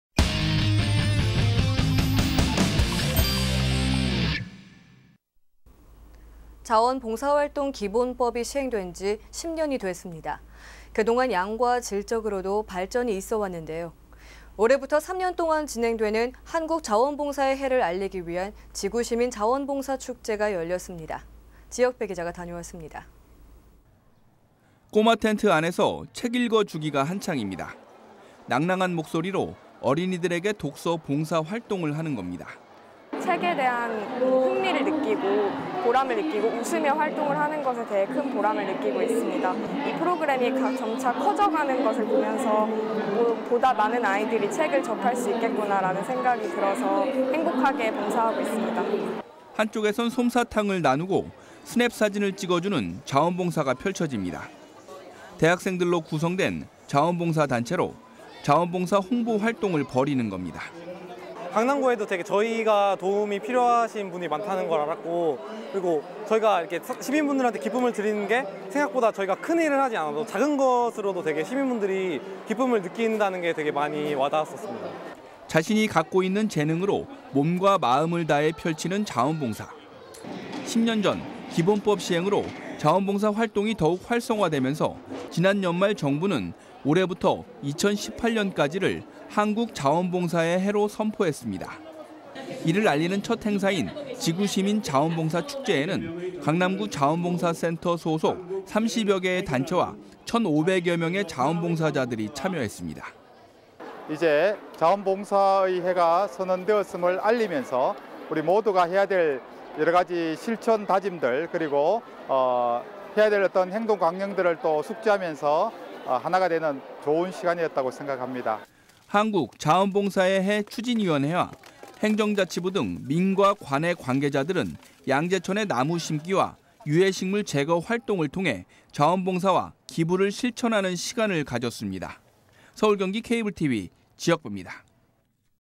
양재천서 '자원봉사 축제'(서울경기케이블TV뉴스)
【 앵커멘트 】
자원봉사활동 기본법이
시행된지 10년이 됐습니다.
그 동안 양과 질적으로도
발전이 있어 왔는데요.
올해부터 3년동안 진행되는
한국자원봉사의 해를 알리기 위한
지구시민 자원봉사 축제가 열렸습니다.
지혁배 기자가 다녀왔습니다.

【 기자 】
꼬마 텐트 안에서
책 읽어주기가 한참입니다.

낭낭한 목소리로 어린이들에게
독서 봉사활동을 하는 겁니다.

【 인터뷰 】
김지안
자원봉사자 / 도곡정보문화도서관
이서연

【 기자 】
한쪽에선 솜사탕을 나누고,
스냅사진을 찍어주는
자원봉사가 펼쳐집니다.

대학생들로 구성된
자원봉사단체로
자원봉사 홍보 활동을 벌이는 겁니다.

【 인터뷰 】
허지원
강남구대학생점프봉사단

【 기자 】
자신이 갖고 있는 재능으로
몸과 마음을 다해 펼치는 자원봉사.

10년전, 기본법 시행으로
자원봉사 활동이 더욱 활성화 되면서
지난 연말 정부는
올해부터 2018년까지를
한국자원봉사의 해로 선포했습니다.

이를 알리는 첫 행사인
지구시민 자원봉사 축제에는
강남구자원봉사센터 소속
30여개 단체와 1천 500여명의
자원봉사자들이 참여했습니다.

【 인터뷰 】
오창섭
센터장 / 한국중앙자원봉사센터

【 기자 】
한국자원봉사의 해 추진위원회와
행정자치부 등 민과 관의 관계자들은
양재천에 나무 심기와
유해식물 제거활동을 통해
자원봉사와 기부를
실천하는 시간을 가졌습니다.

서울경기케이블TV 지혁뱁니다.